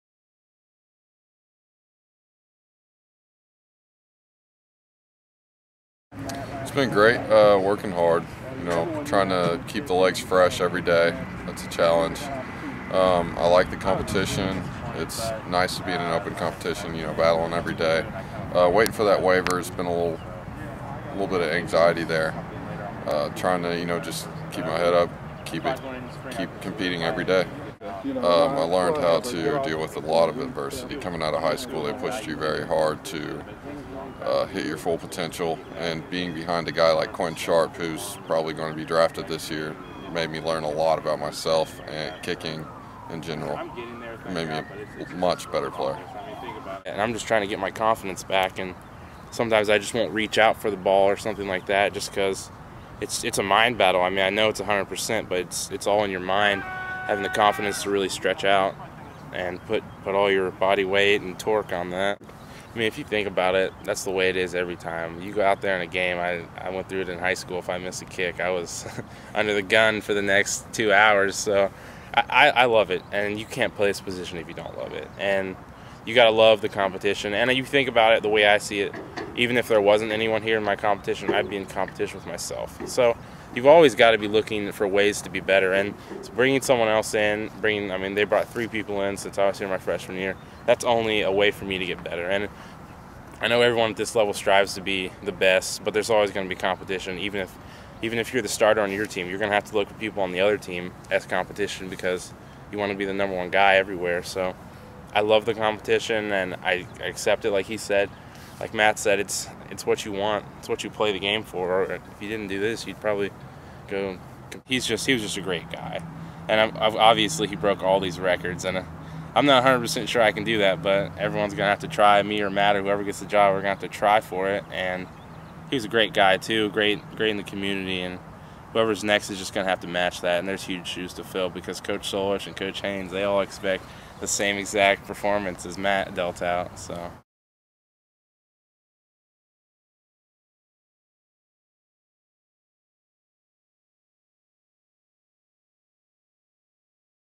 2013 FB | Spring Practice - Day Eight
Ohio kickers Matt Green and Josiah Yazdani stop by to chat with OhioBobcats.com to discuss the Bobcats' kicking game.